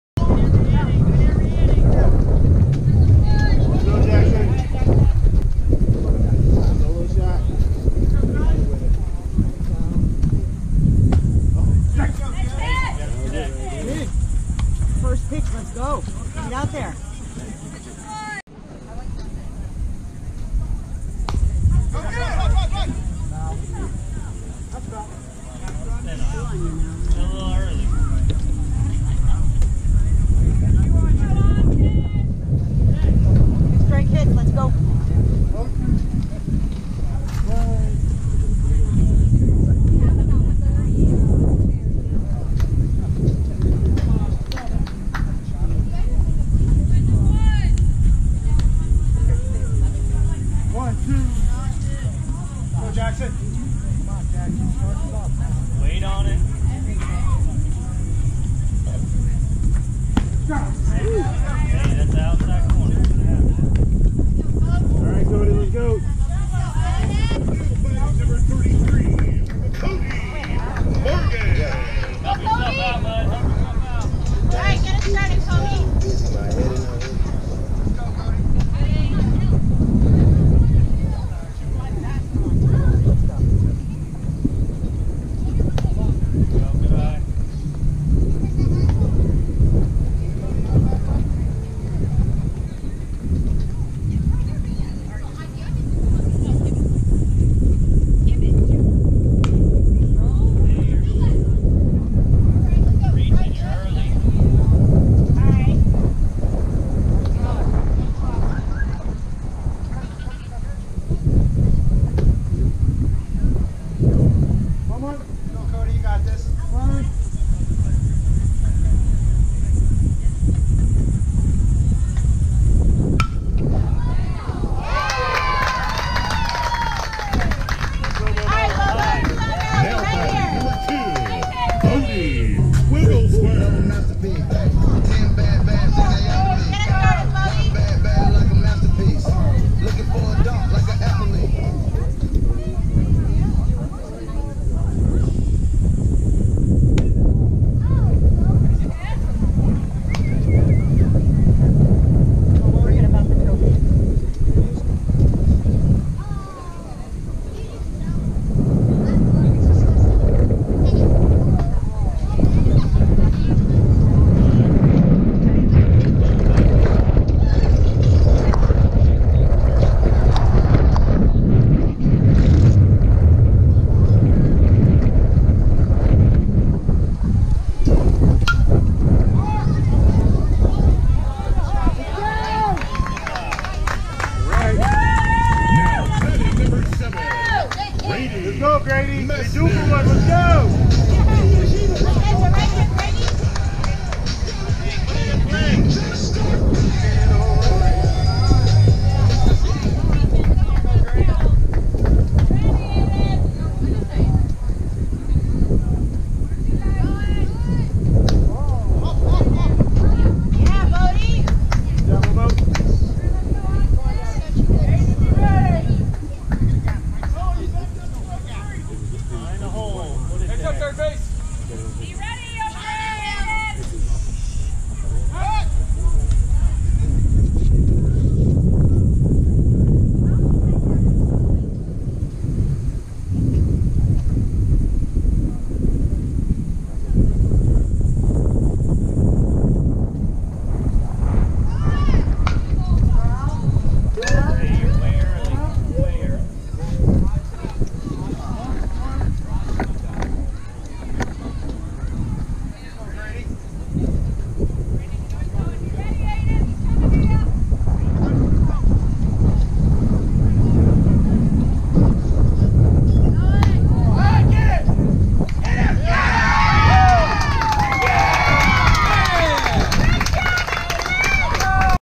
Hurricanes vs Vero Beach Crush - Inning 4 Bottom
Inning 4 Bottom - Hurricanes at Bat
Summer 2020 Last Dance Vero Beach
Game 5 - Championship Game - 08/23/2020